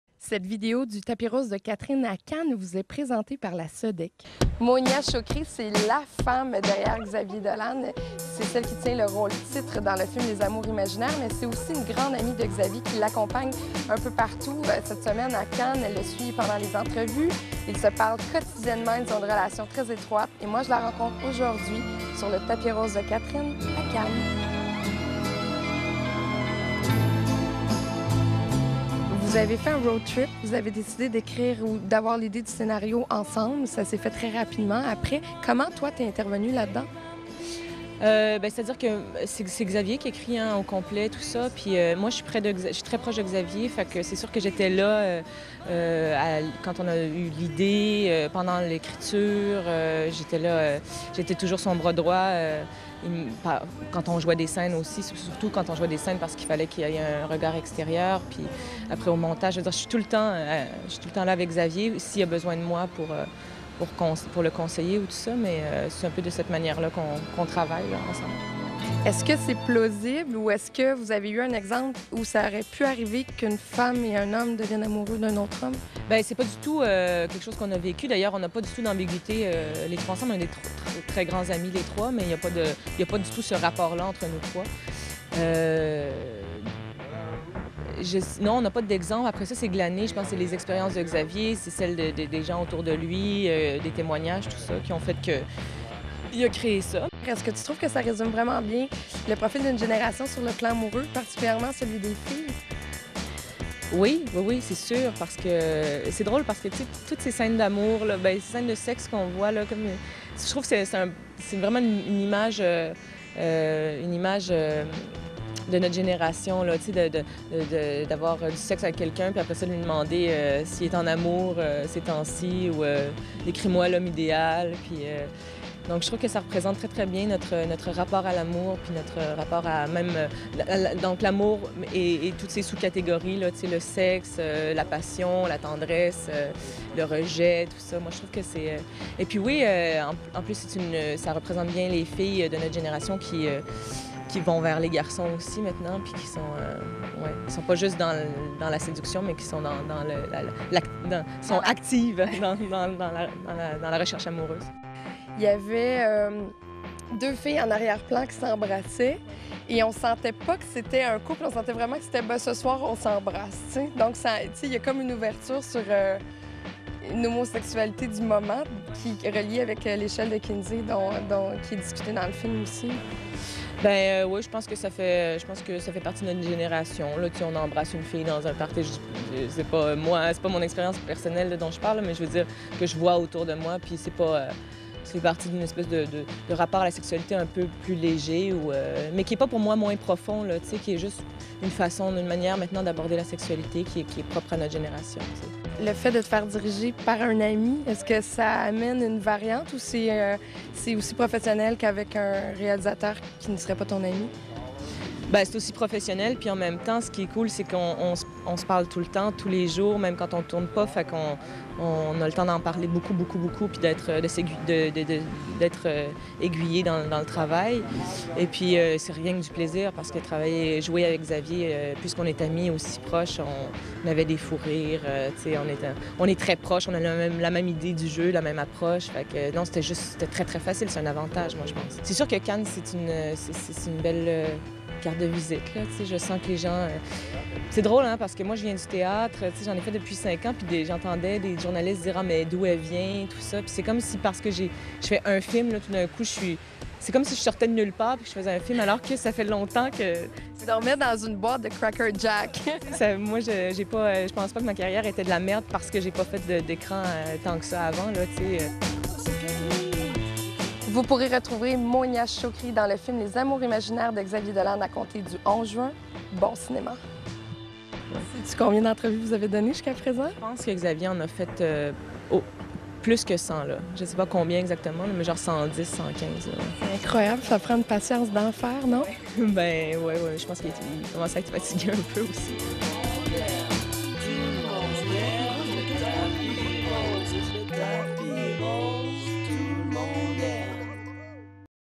Monia Chokri-Les amours imaginaires
Le tapis rose de Catherine rencontre Monia Chokri à Cannes pour le film de Xavier Dolan en compétition officielle au Festival de Cannes